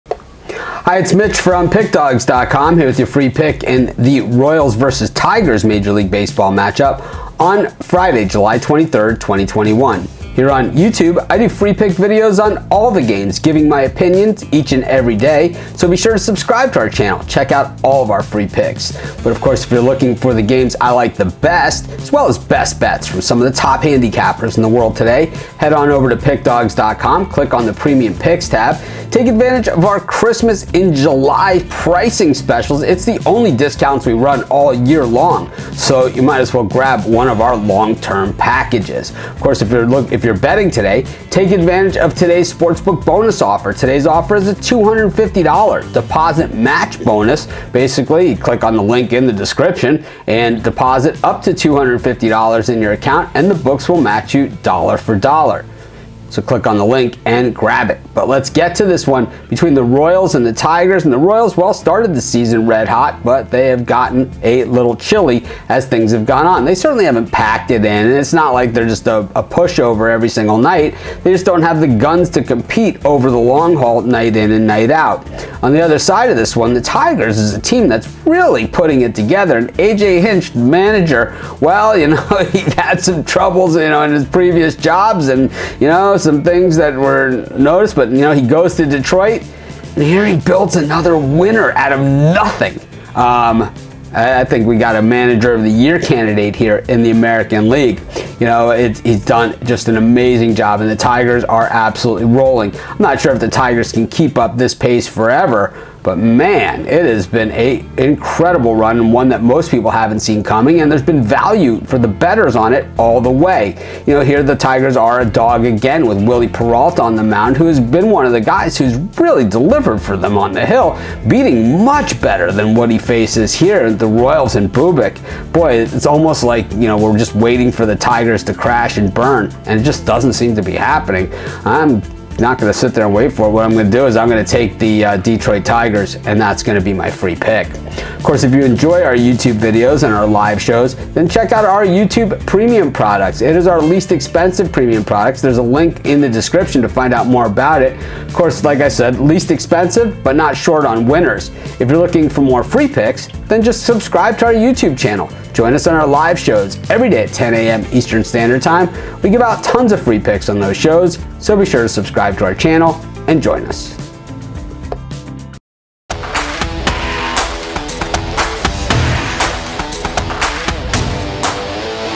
MLB Pick Kansas City Royals vs Detroit Tigers 7/23/21 MLB Betting Pick and Prediction
The Detroit Tigers face off with the  Kansas City Royals in Friday's MLB Baseball action. Get Mitch's Expert Pick and Prediction for this game.